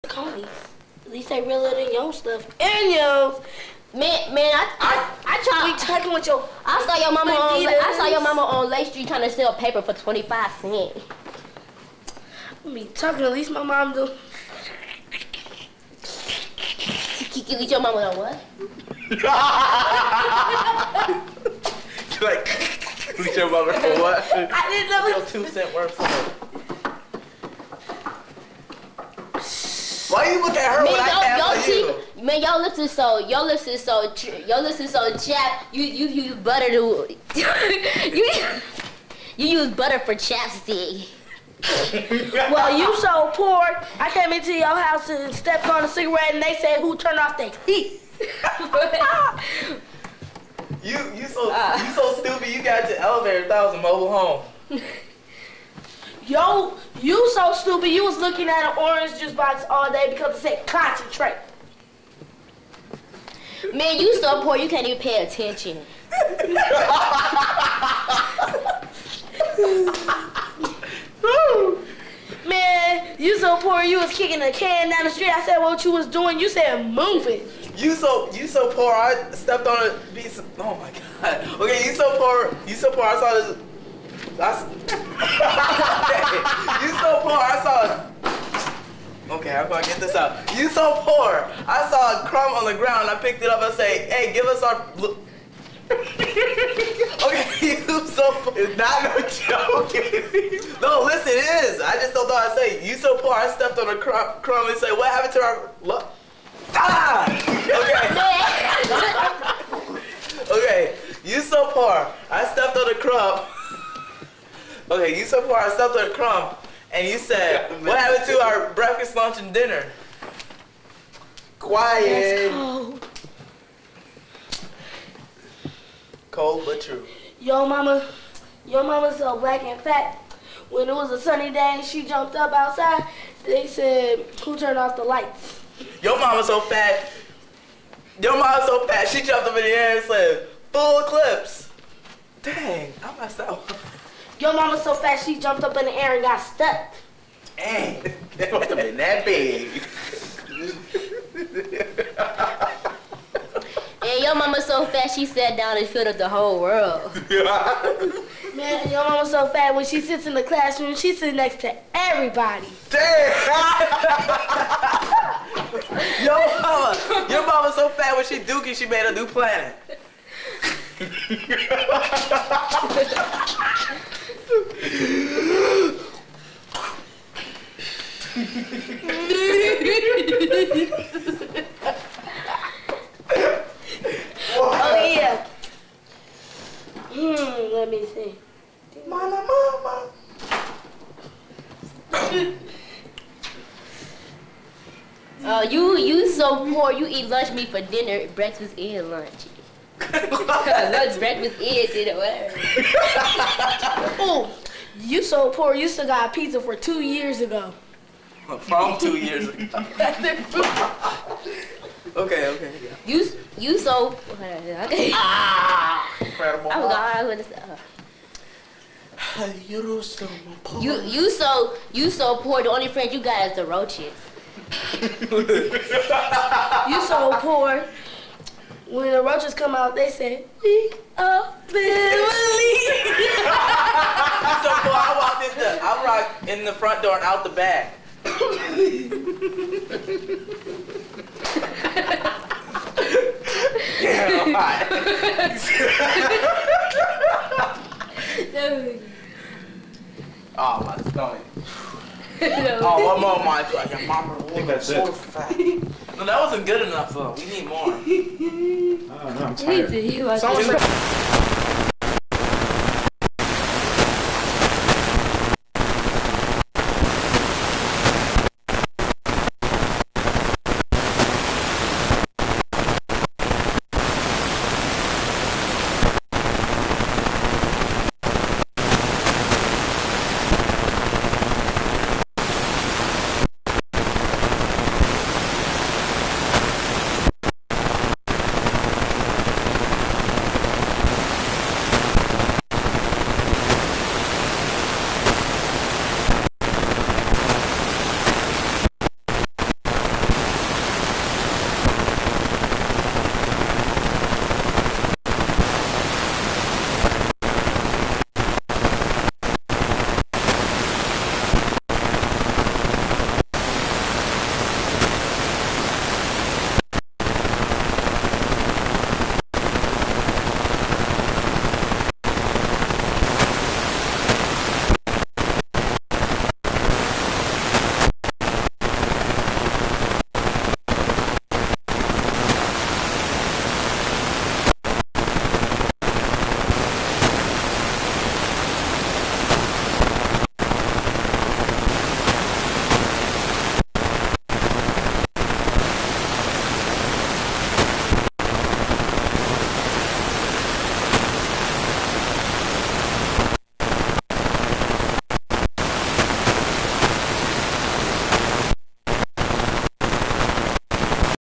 PCTV Our Turn Crew 734 E Lake St
Brandy and Amanda (and James and Cedric???)
do the dozens. i suppose this was some sort of

pre-production research.